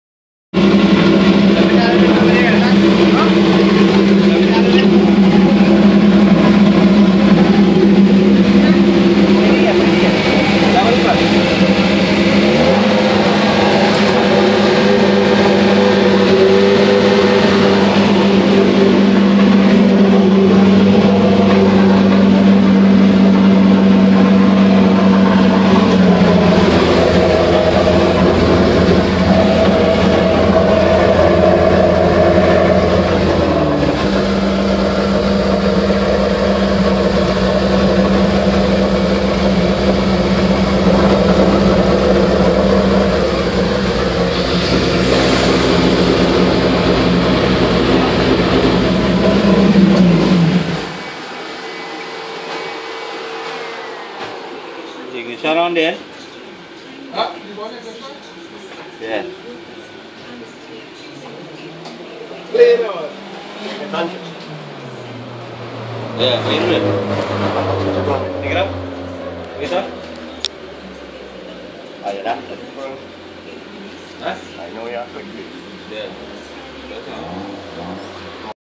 Racecar first ever startup
First ever start up of Nissan B13 race car with SR16ve  and ITB's ,this was back in 2013 before we had better camera footage